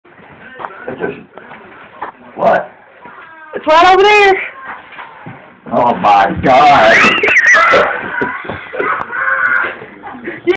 The Purple Surprise
we decided to play a joke on my dad by putting a purple dildo on the shower wall.